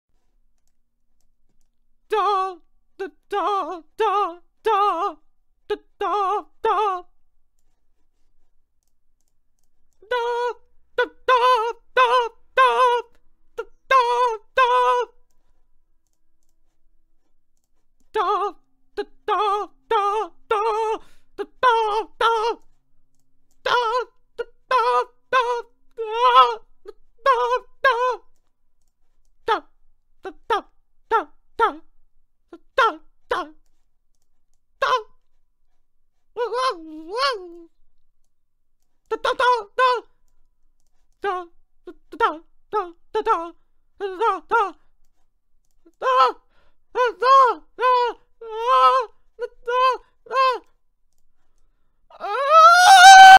dah
sorry for the long intro guys video starts at 00:52

i was just planning on drawing some beautiful art when suddenly my computer gets possessed by this operatic demons

I am livid with these demons. You can't put a price on manners but i tell you what I want these demons to pay like four pounds and a bag of crisps or if you live outside the UK a stick and a bag of marbles

if someone could caption this I'd be so grateful

Love, bbygurl92xox